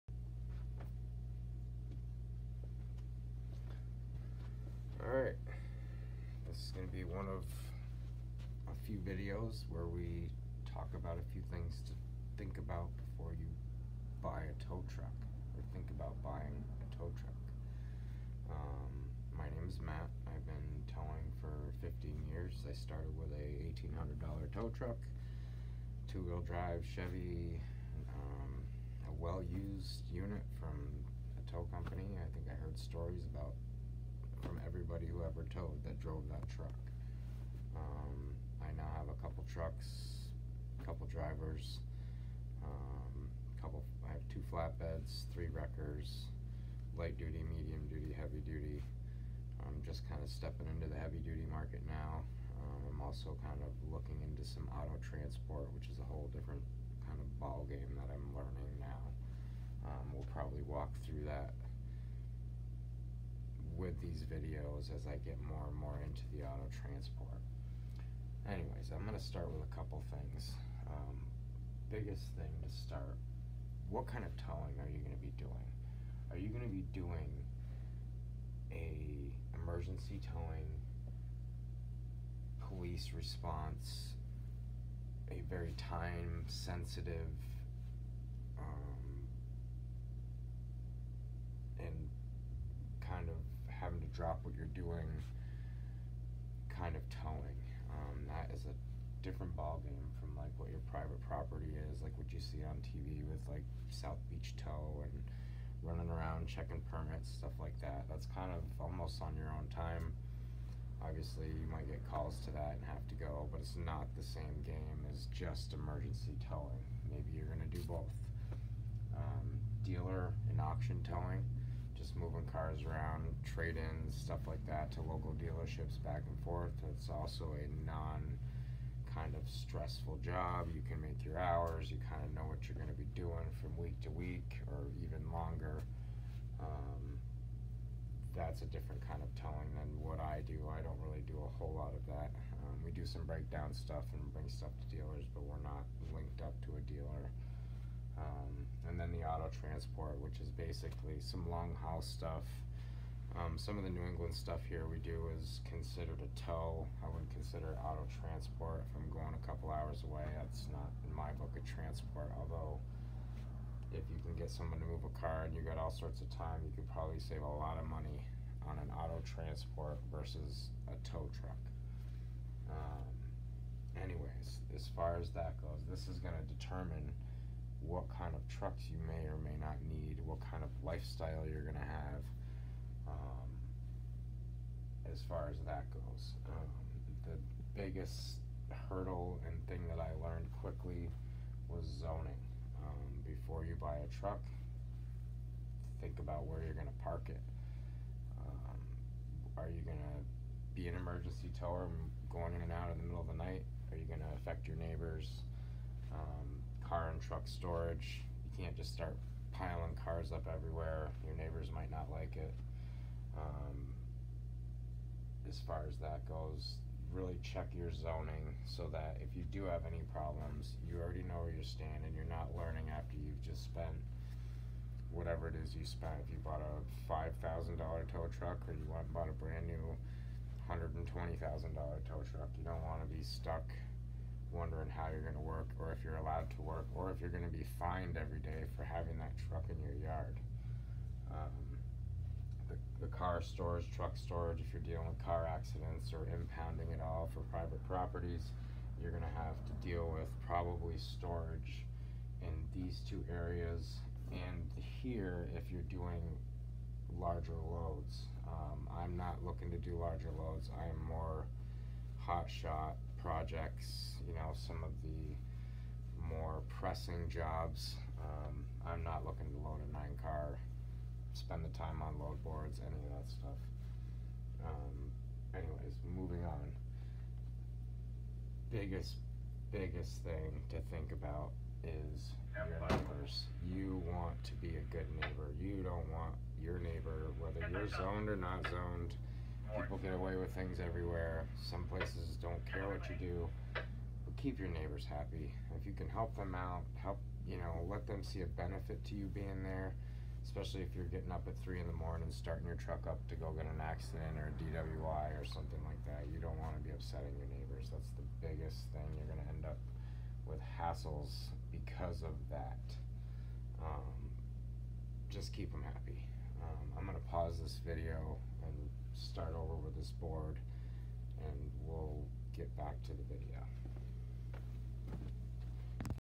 What you need to know about towing before buying a Tow Truck. Part 1.
A video series I’m putting together about Towing, things to think about, and my person experiences and “opinions”.